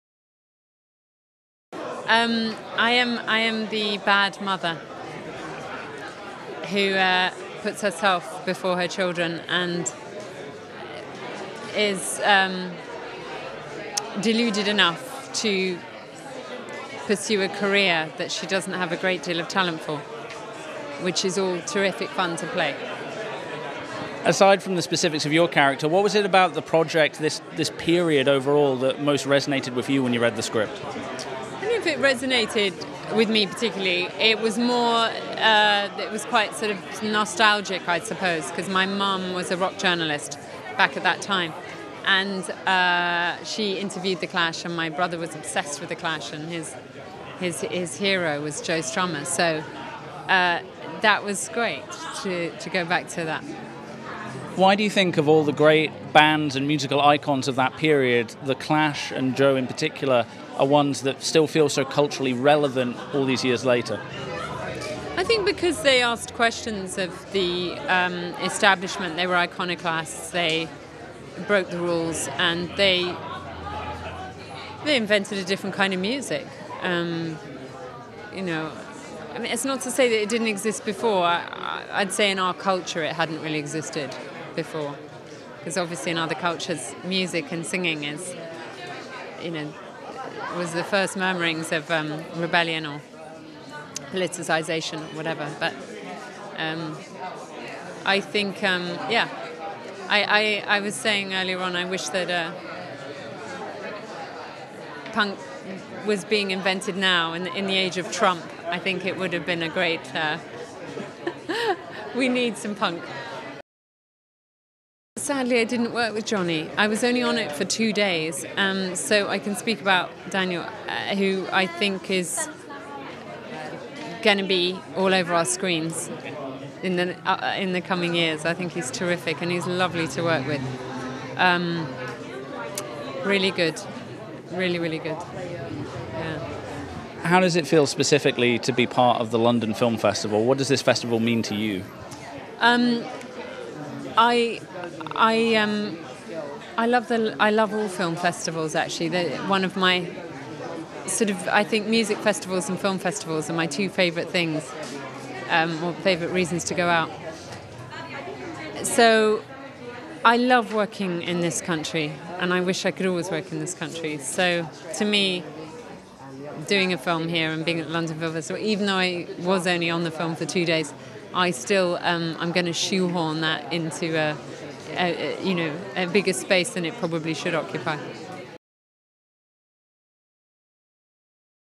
Natascha McElhone London Town Premiere
Red Carpet News TV talks to Jonathan Rhys Meyers, Natascha McElhone, Dougray Scott, Daniel Huttlestoneand director Derrick Borte at the BFI London Film Festival premiere for musical drama London Town. The film follows a 14 year old boy’s adventures as he forges an unlikely friendship with a young rock singer who turns out to be iconic front man Joe Strummer from The Clash.